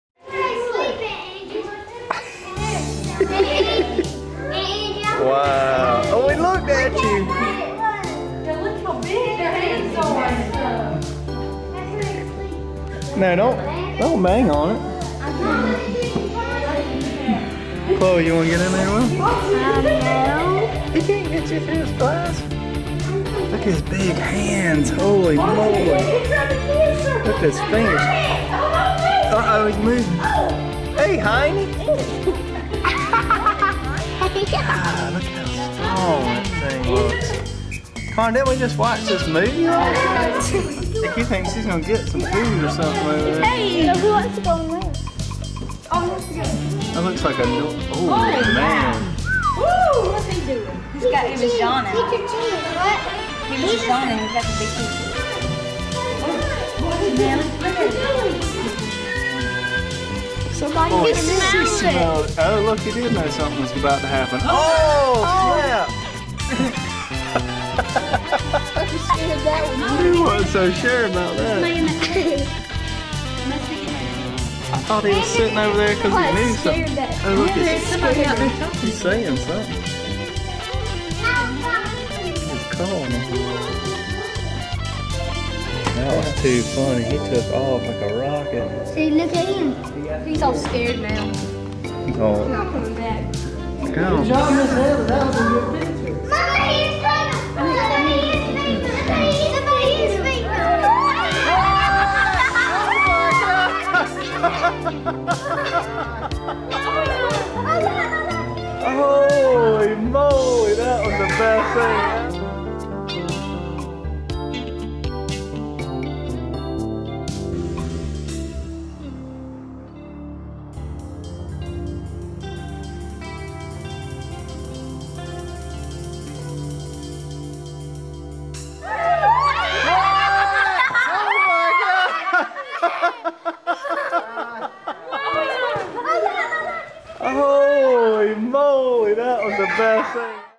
Gorilla Walks/Runs Upright Like a Man (long)
Rediscovered this golden home movie footage from a trip to the Riverbanks Zoo in Columbia, SC 2008!